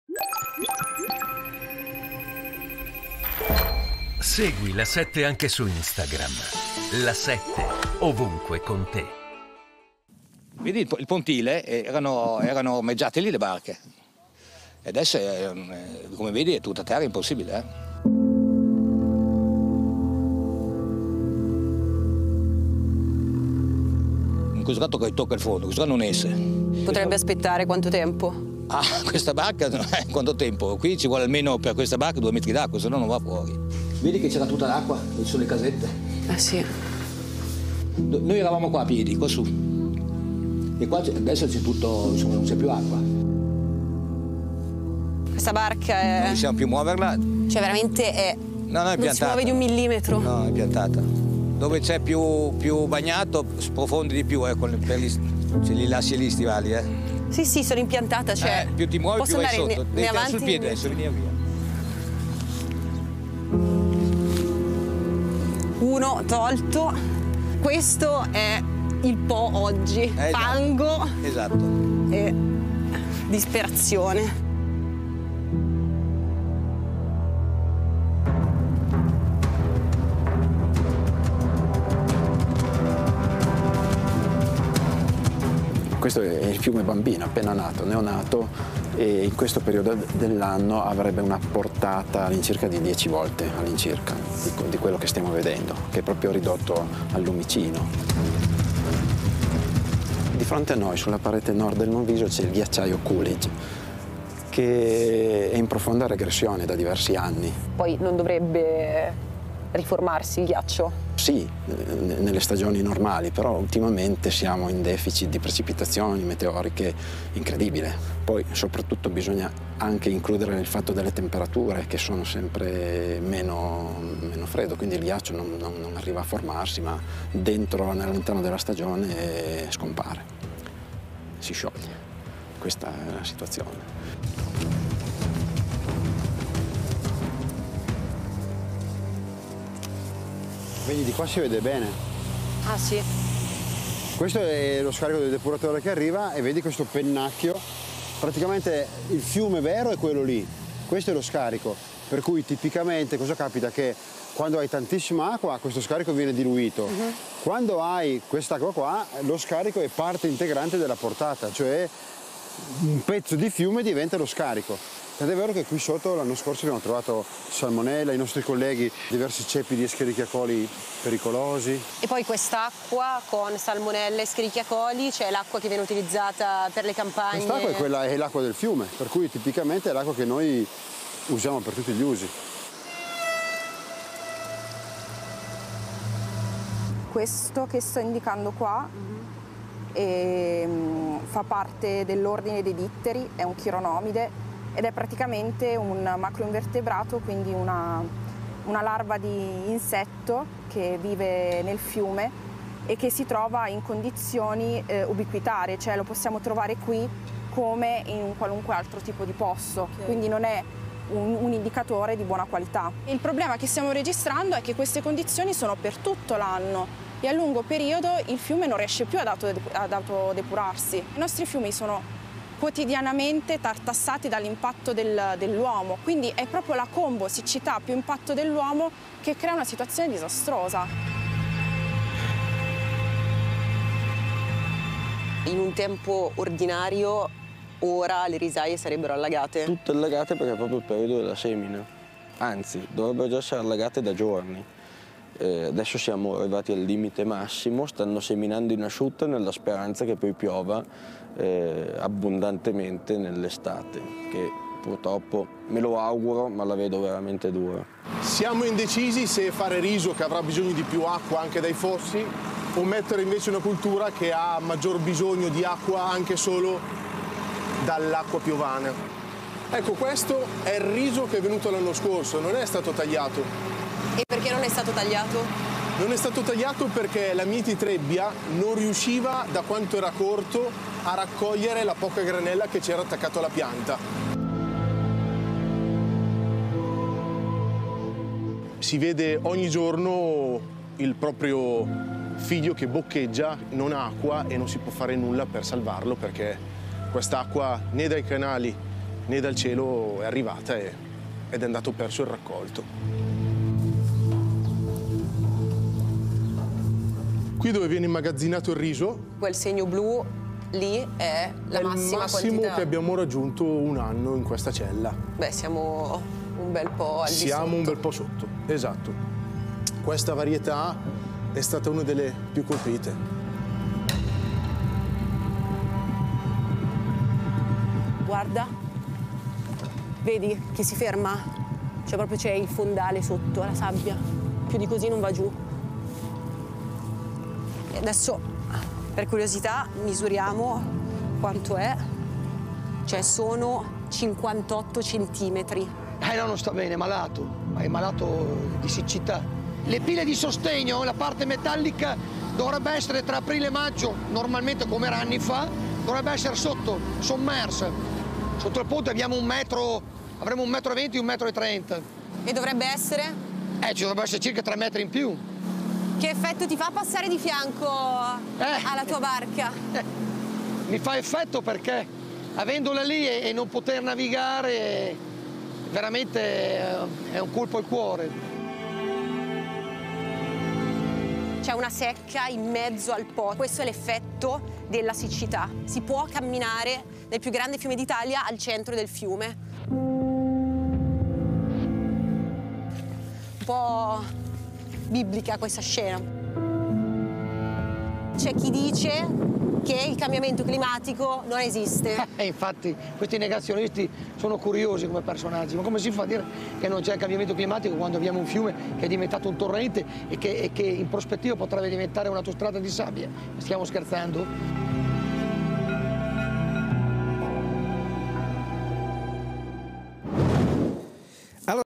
Po, il grande fiume malato di siccità
Barche sull'erba. Fondali bassissimi e secche. Ghiacciai in regressione. La siccità del maggiore fiume d'Italia in un viaggio dal Piemonte all'Emilia Romagna. Il reportage di Beatrice Broglio